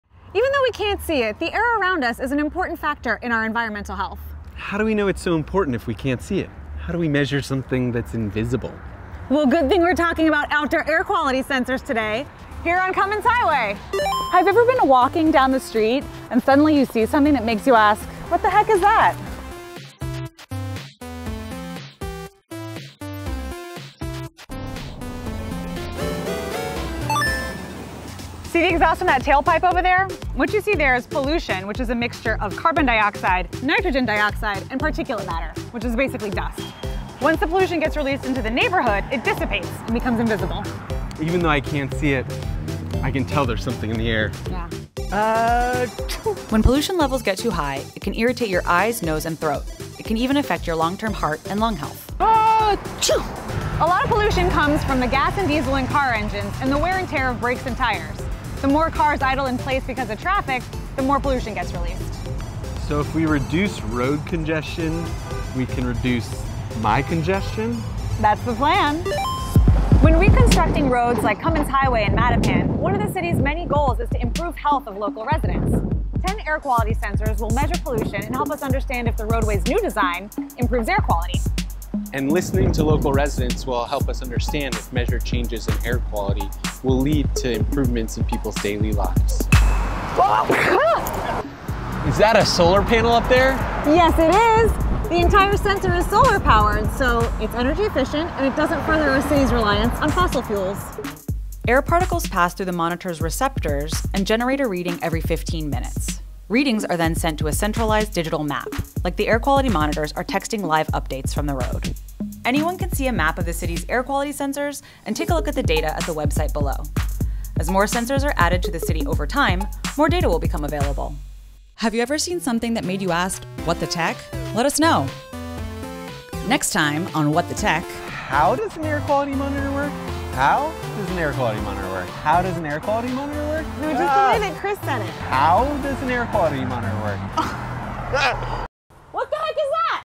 What The Tech: Episode 7 - Air Quality Sensors
How do you measure something that is invisible? Join us as we walk Cummins Highway in Mattapan to look up to the sky and ask "What the Tech is that?"